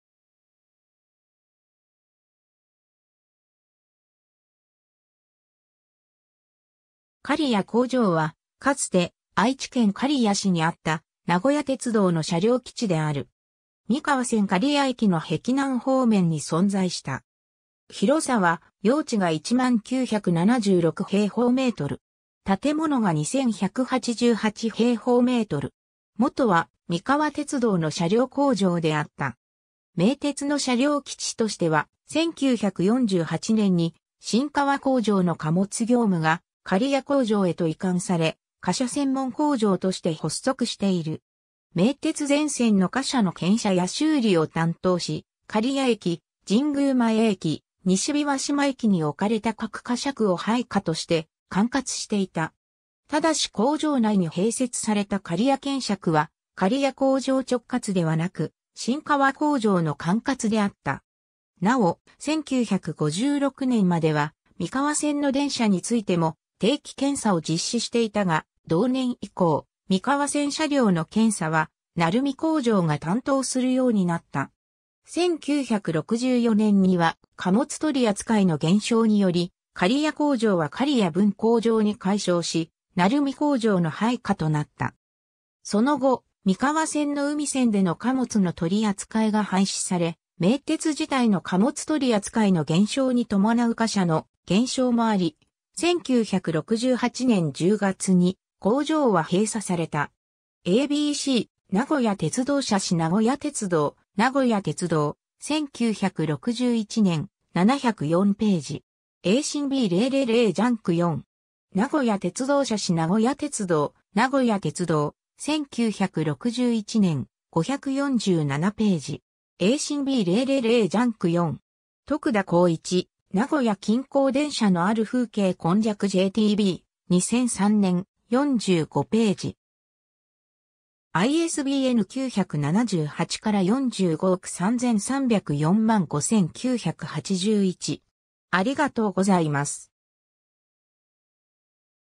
名古屋鉄道刈谷工場
刈谷工場（かりやこうじょう）は、かつて愛知県刈谷市にあった、名古屋鉄道（名鉄）の車両基地である。
三河線刈谷駅の碧南方面に存在した。
広さは用地が10,976m2。
建物が2,188m2。
 元は三河鉄道の車両工場（刈谷車庫）であった。
名鉄の車両基地としては、1948年（昭和23年）に新川工場の貨物業務が刈谷工場へと移管され、貨車専門工場として発足している。
名鉄全線の貨車の検車や修理を担当し、刈谷駅、神宮前駅、西枇杷島駅に置かれた各貨車区を配下として管轄していた。
ただし工場内に併設された刈谷検車区（かりやけんしゃく）は刈谷工場直轄ではなく、新川工場の管轄であった。
なお、1956年（昭和31年）までは三河線の電車についても定期検査を実施していたが、同年以降、三河線車両の検査は鳴海工場が担当するようになった。
 1964年（昭和39年）には貨物取扱の減少により刈谷工場は刈谷分工場に改称し、鳴海工場の配下となった。
その後、三河線の海線での貨物の取り扱いが廃止され、名鉄自体の貨物取り扱いの減少に伴う貨車の減少もあり、1968年（昭和43年）10月に工場は閉鎖された。
 ^ a b c 『名古屋鉄道社史』名古屋鉄道、名古屋鉄道、1961年、704頁。
ASIN B000JAMKU4。
 ^ 『名古屋鉄道社史』名古屋鉄道、名古屋鉄道、1961年、547頁。
ASIN B000JAMKU4。
 ^ 徳田耕一『名古屋近郊電車のある風景今昔』JTB、2003年、45頁。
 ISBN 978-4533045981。